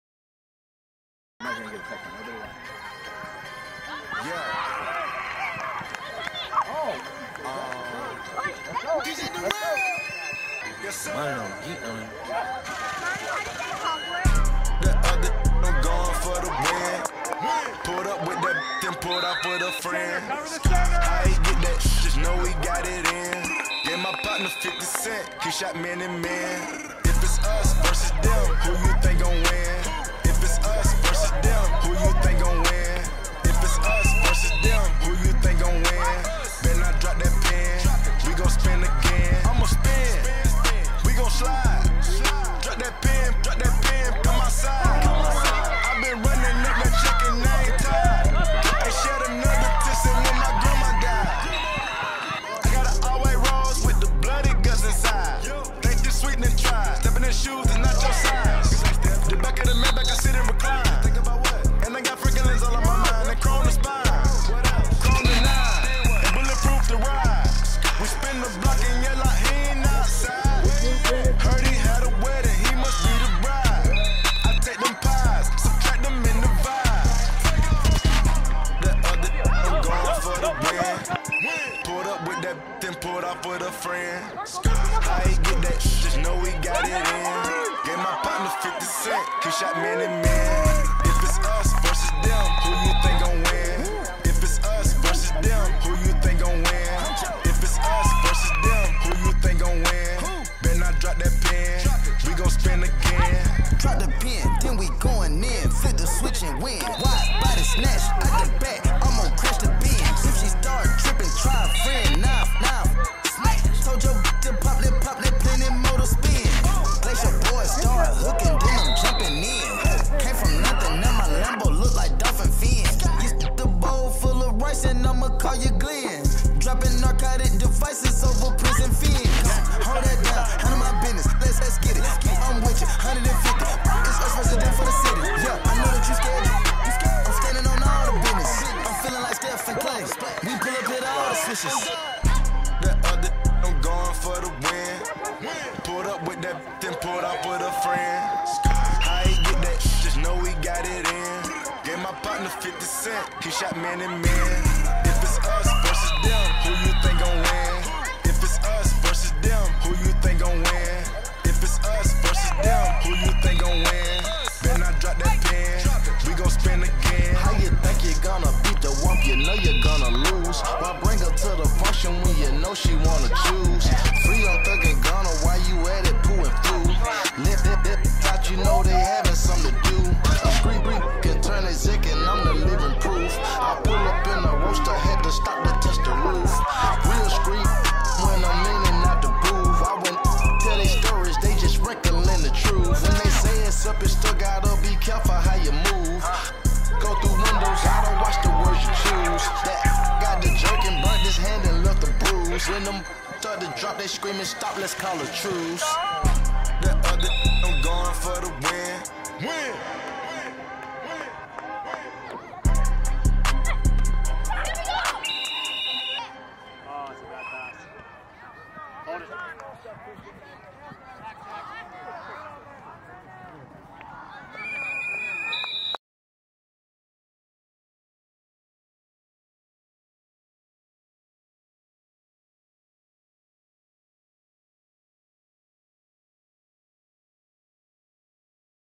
Volunteers Vs. Tigers (20-7 Victory)
The 5th/6th Grade Volunteers took on a solid Tigers team. Not to be confused with the Tigers from week 1, This game brought fireworks and suspense as the Volunteers took late control and ended on top 20-7.